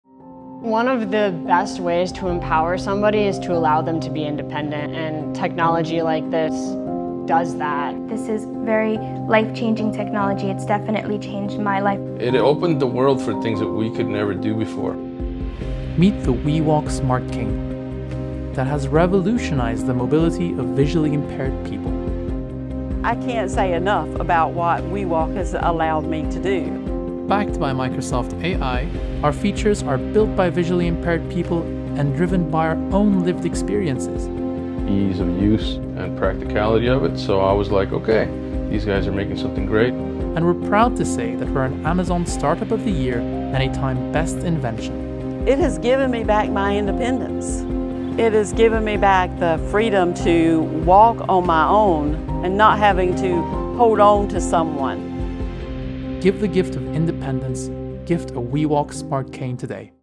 WeWALK Together for the Independence of Visually Impaired People
Give the Gift of Independence to a Visually Impaired Person,
Gift WeWALK Smart Cane Today!

There are 253 million visually impaired people in our world today, and we want every individual to experience the ultimate mobility. Together, we can truly empower the independence of countless people living with visual impairments.

Every dollar raised on our platform will go directly towards purchasing WeWALK Smart Canes for deserving individuals across the visually impaired community.

to donate directly to the individual fundraisers to contribute to the organizations that empower tens of thousands of people who are blind or visually impaired.